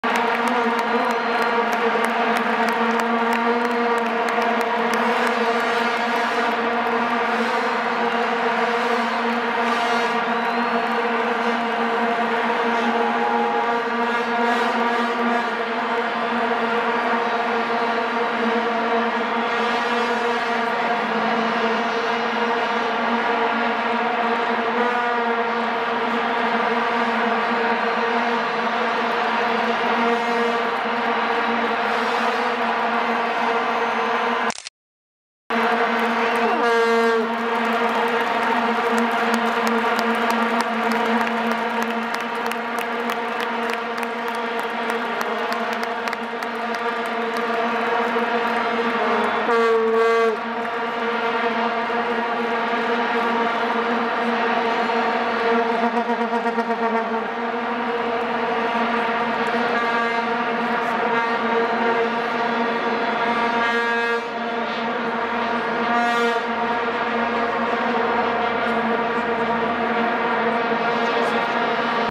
Mandela na final do Mundial de futebol 2010
Nelson Mandela no estádio Soccer City saudando aos presentes no encerramento do Mundial 2010